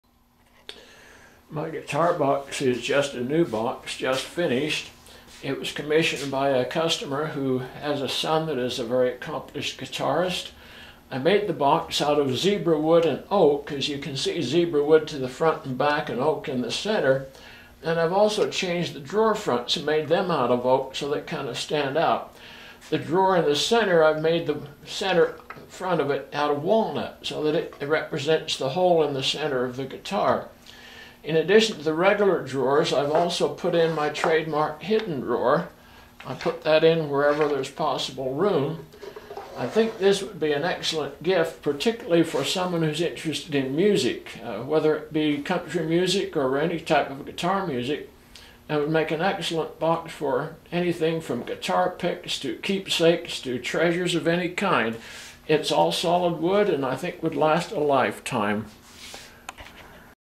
Guitar Shaped Jewelry Box
ETSY - COMMISSIONED JEWELRY BOXES

Our new guitar band saw box was commissioned by a good customer as a gift for her son who plays both the 6 and 12 string acoustic guitars as well as the classical guitar. I patterned this box from a picture of his classical guitar. The box has 2 large drawers plus a small drawer representing the hole in the center of the guitar. It also has a hidden drawer. This unique box would be special to anyone interested in music, especially country music or any type of guitar music.